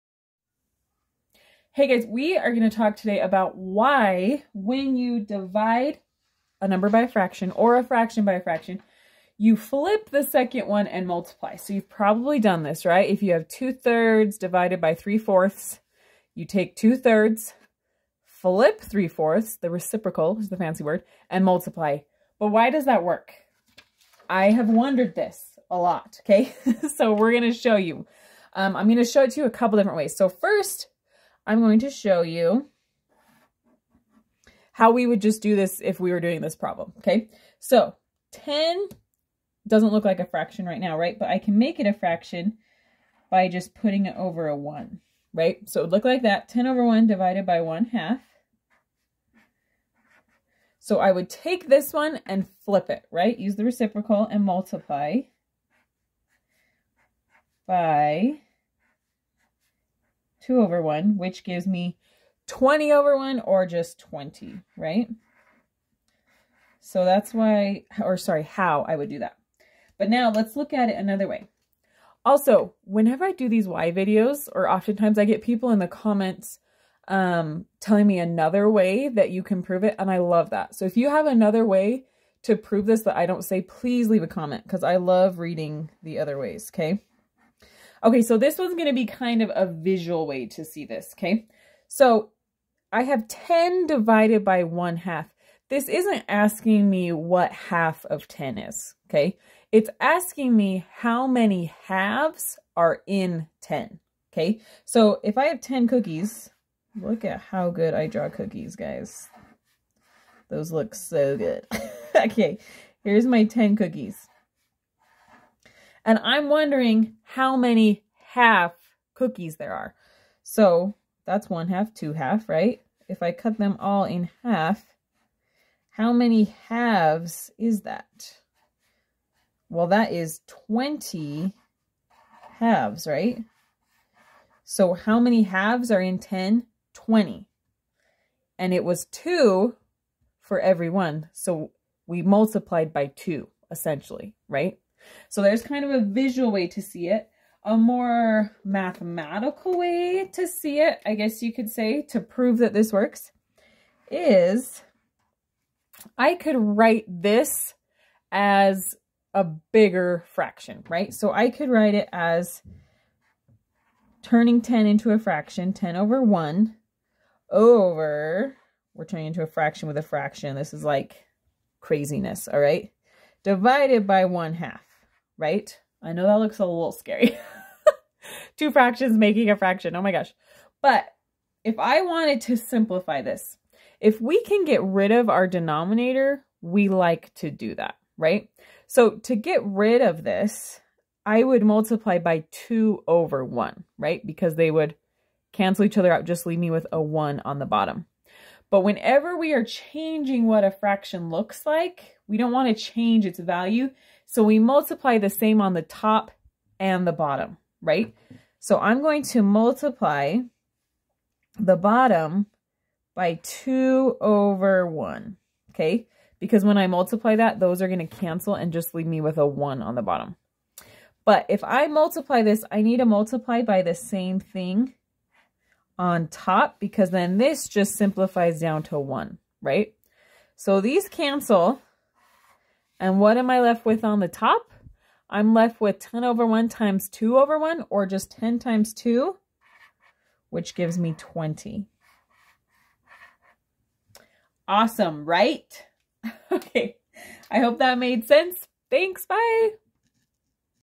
Why Do We Flip the Second Fraction & Multiply to Divide Fractions?
When we divide fractions, you just flip the second fraction (fancy word: reciprocal) and multiply. But why do we do this? Why does this work? In this video, you'll see a simple division problem with a fraction done a few different ways to show why this works.

In math, there are usually multiple ways to explain things.